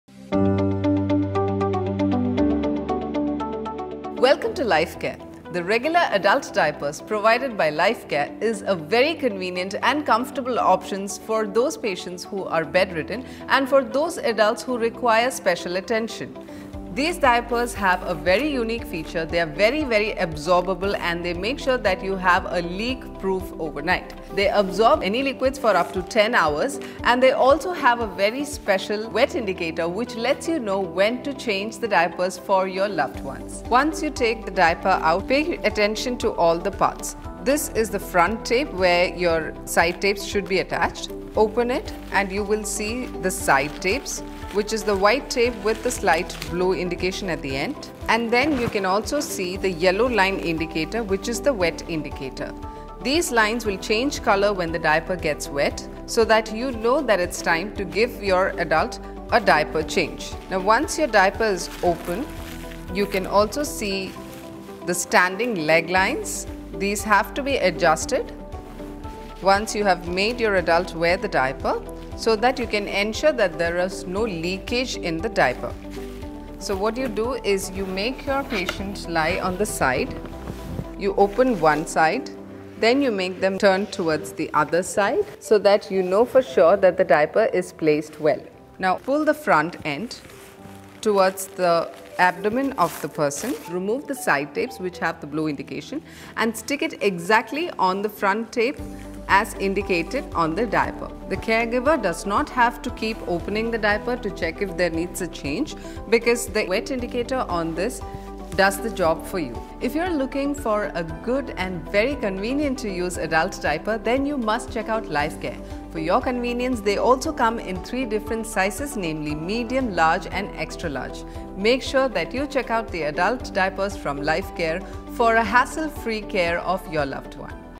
HOW TO USE ADULT DIAPERS
ADULT DIAPERS

welcome to life care ..Adult diapers provided by life care are comfy and convenient..
When replaced timely it keeps skin dry and comfortable

• let the patient lie on his side
• Open the diaper and make the hind part with tape facing upward
• turn the patient to the other side
• open the other side of the diaper in the same manner
• let the patient lie on his back, pull the frontal tapes to the belly and fasten it       in the correct area
• flexible pleats can be adjusted for better fit